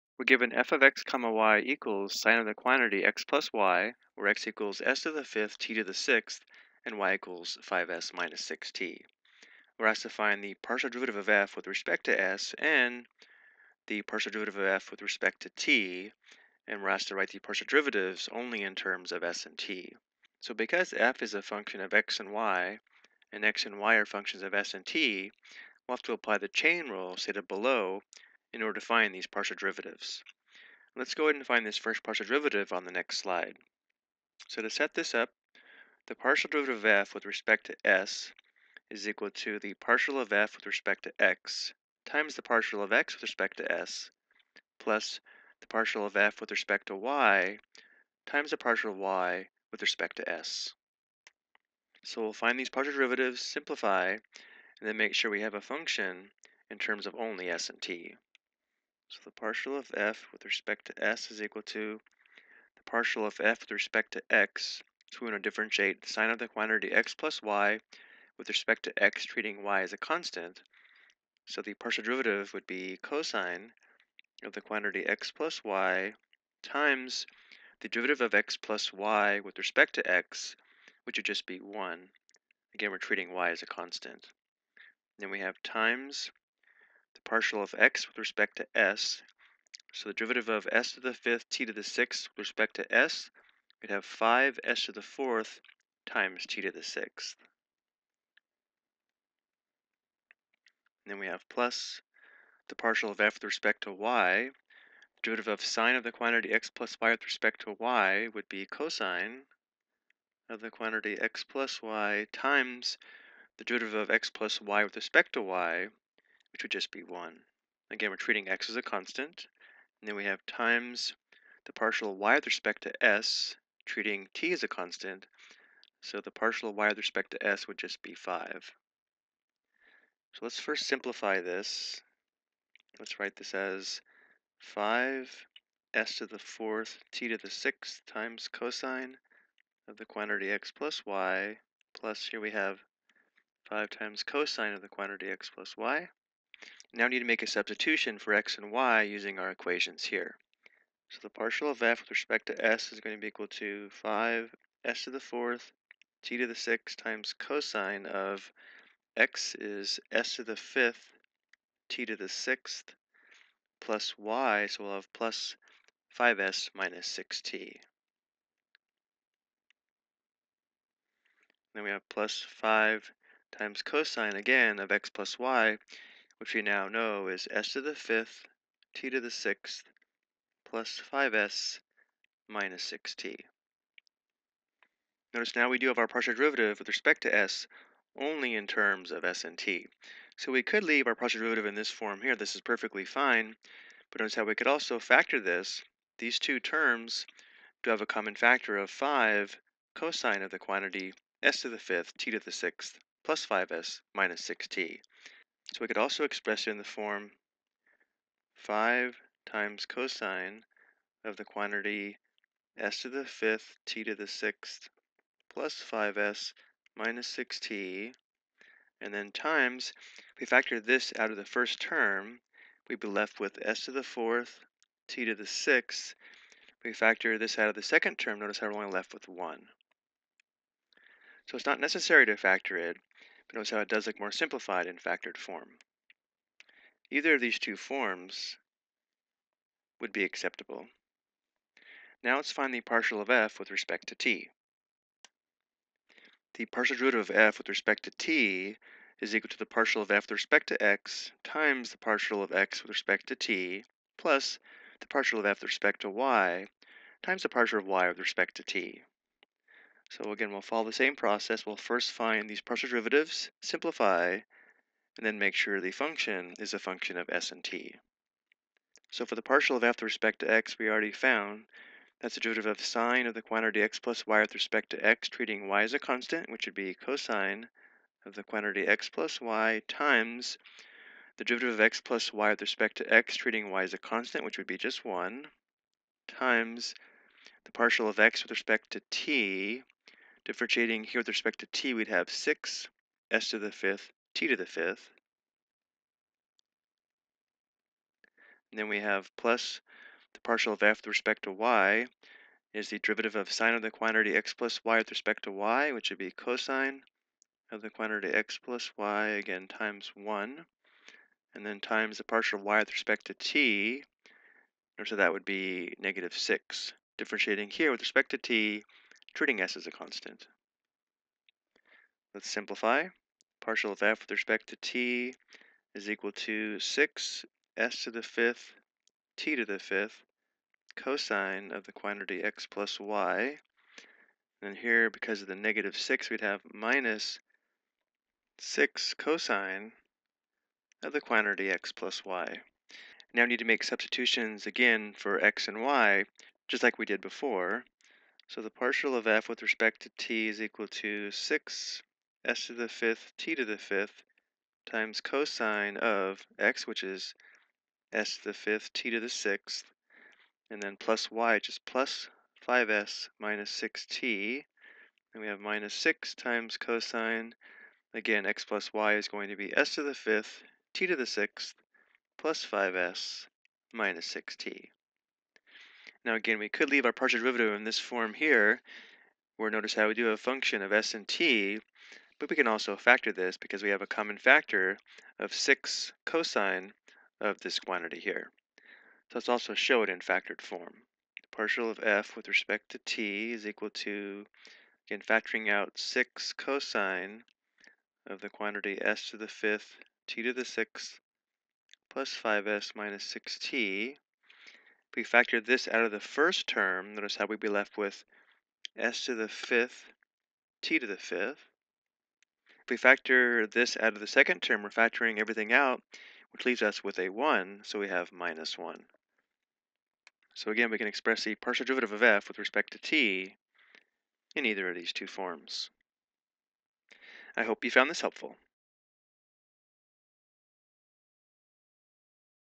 Ex: Chain Rule - Function of Two Variables with Two Independent Variable
This video explains how to determine a partial derivative of  a function of two variables using the chain rule.